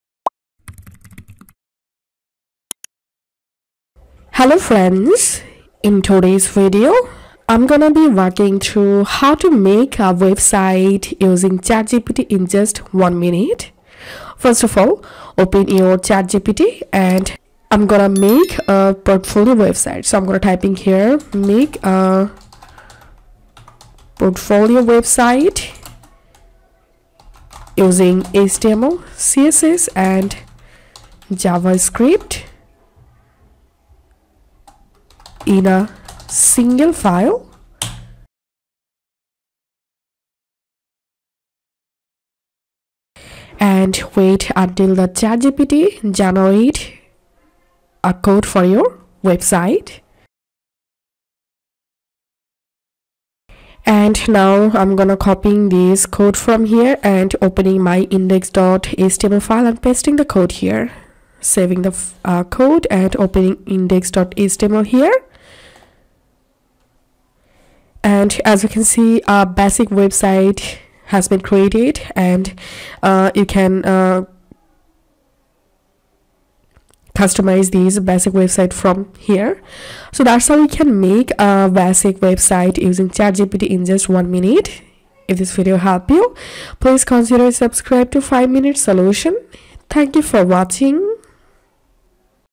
Build a Website using ChatGPT in 1 Minute | Make website with ChatGPT
Welcome to the exciting world of natural language processing and machine learning! Today, we will be exploring the capabilities of ChatGPT, a state-of-the-art language model developed by OpenAI. One of the most impressive features of ChatGPT is its ability to generate source code based on simple descriptions. Imagine being able to build a complete website in just one minute without writing a single line of code yourself. Sounds too good to be true? Let's find out!